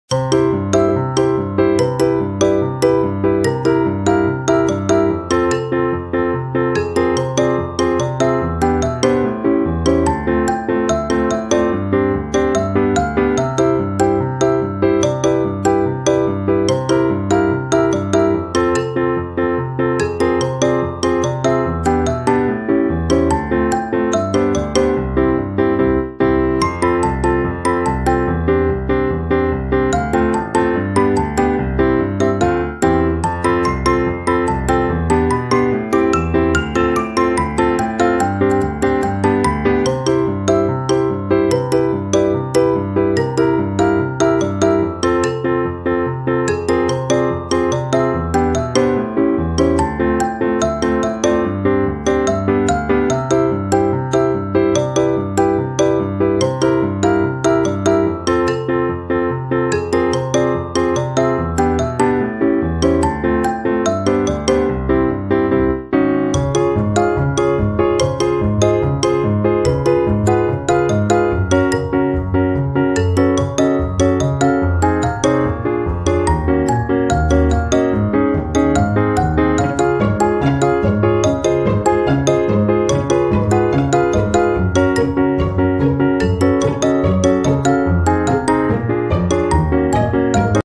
waste diya reuse idea ll best out of waste diya ll Diwali craft idea ll DIY ll Home decoration ❤❤🔥
Hello friends.
In this vedio i will show you how to reuse Diwali diya and how to decor home

Material
reuse diya
cardboard
gota
mirror
favicol